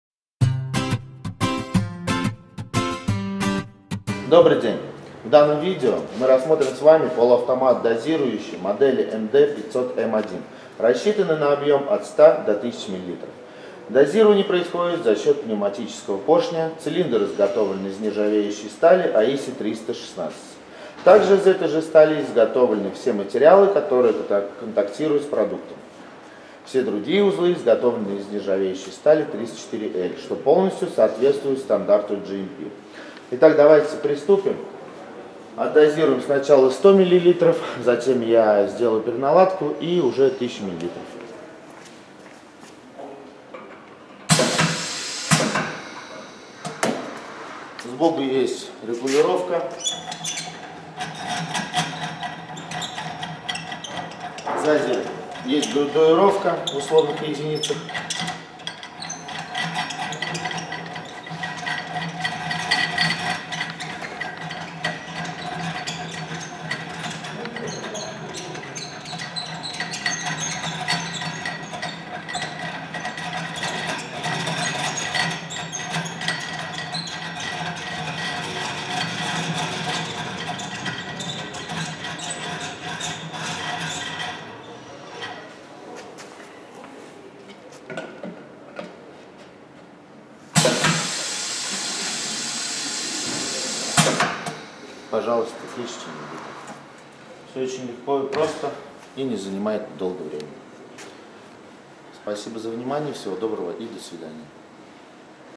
Полуавтоматический дозатор МД-500М1.
В данном видео продемонстрирован процесс дозирования жидкости в тару объемом 100 мл и 1000 мл на Полуавтоматическом дозаторе МД-500М1.

Описание:

Полуавтоматический объемно-поршневой дозатор МД-500М1 с приемной емкостью предназначен для дозированной подачи жидких, вязких, густых, пастообразных продуктов с минимальными вкраплениями во флаконы. Изделие может применяться в фармацевтическом и химическом производствах.
Дозатор использует поршневой принцип розлива. Для отсекания продукта и предотвращения эффекта «последней» капли используется пневматический клапан.

С более подробным описанием данного оборудования Вы можете ознакомиться на нашем сайте по ссылке

Характеристики:

Количество дозирующих сопел: 1 шт;
Вес: 30 кг;
Габариты, мм (Д х Ш х В): 806 х 180 х 690;
Производительность: 1000 шт/ч;
Напряжение: 220;
Тип дозатора: Полуавтоматические поршневые дозаторы;
Тип продукта: Лекарственный препарат, Вода и негазированные напитки, сок, агрессивная жидкость;
Диапазон розлива: 10-5000 мл;
Тип тары: Стеклянная тара, Пластиковая тара;
Отрасль: пищевое производство, косметика, электронные сигареты;